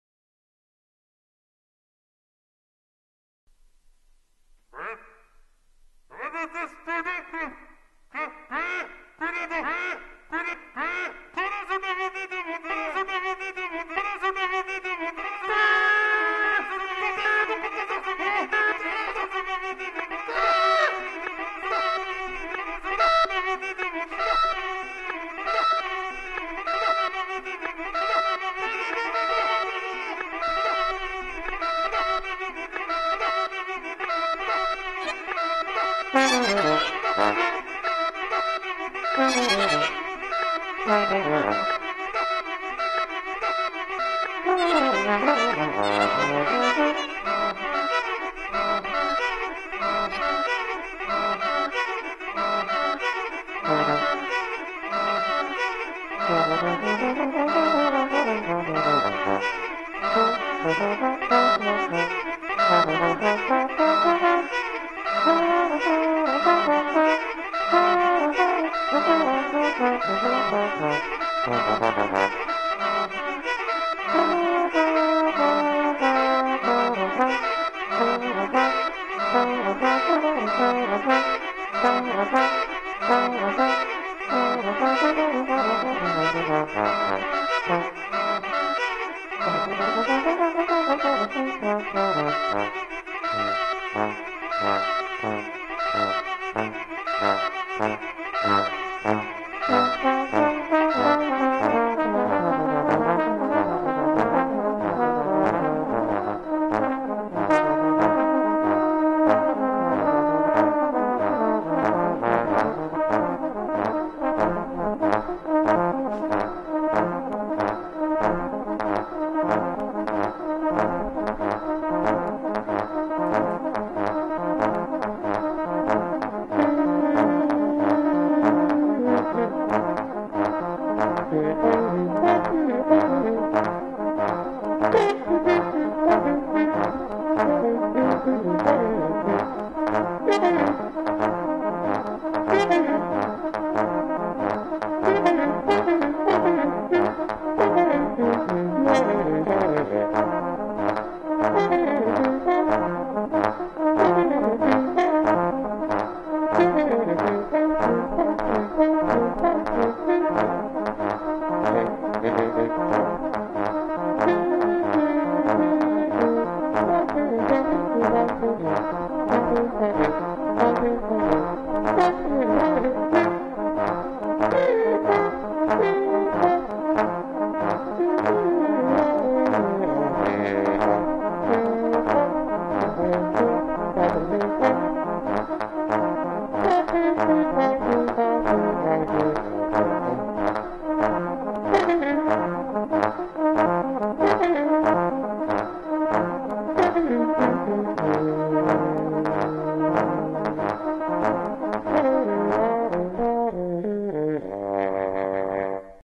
Meta-trombone concert in Paris (July 2013)
Playing my meta-trombone at the 2013 Paris Loop Jubilee.

Improvised music for trombone and algorithms.

(trombone + live looping)